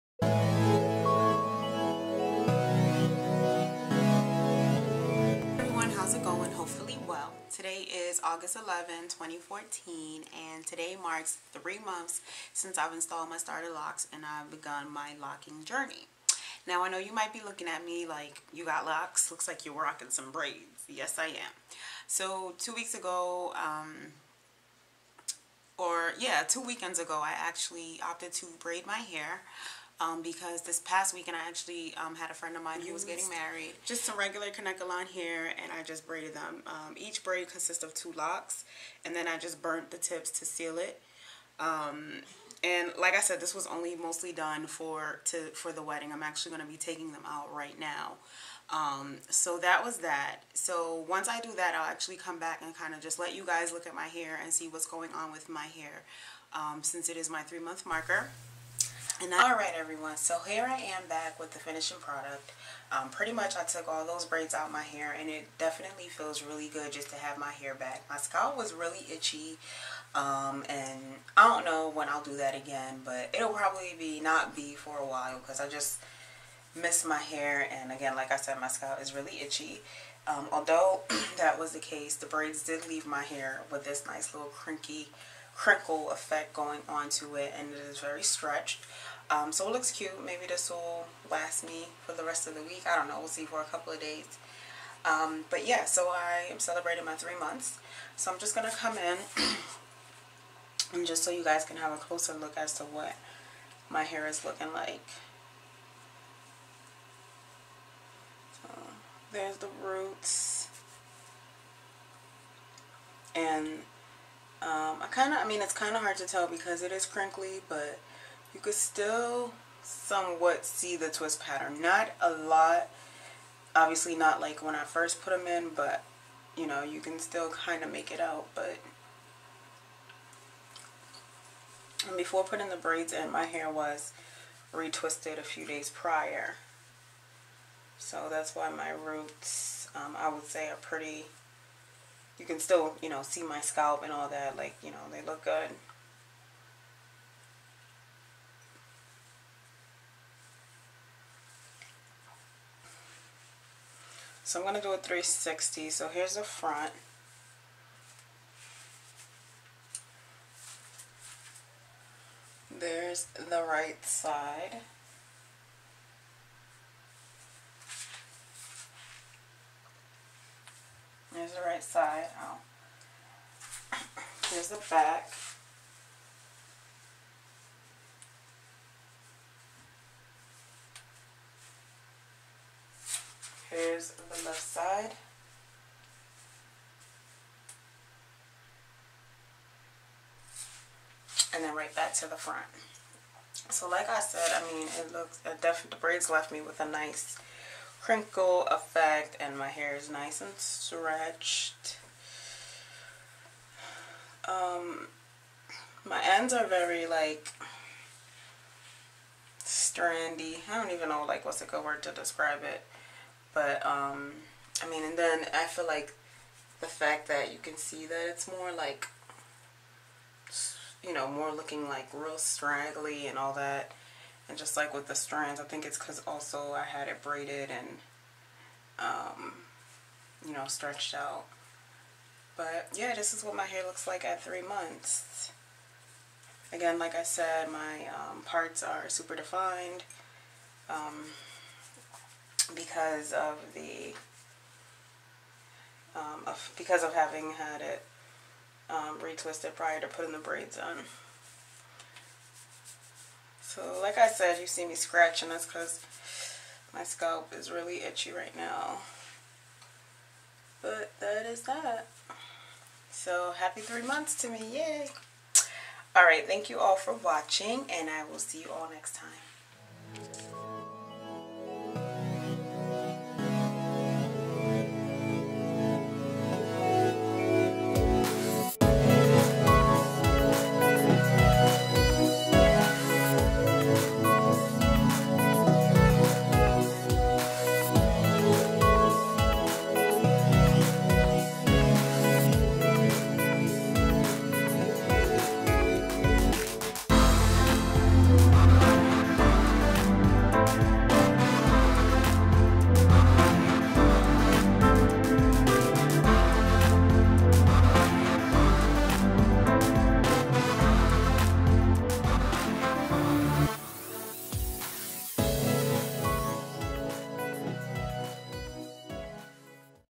3 Month Loc Update
(Recorded August 11, 2014) I am now 3 months into my Loc Journey and my hair seems to be progressing well.  I haven't experienced many changes this past month. My hair continues to bud and shrink.  I included a few close up pics at the end of the video, so take a look and tell me what you think.

Braids:
After taking out my braids, my hair was left stretched and with crinkles.  My scalp was really itchy and my hair looked a bit dry. Not too sure when I will be braiding my locs again.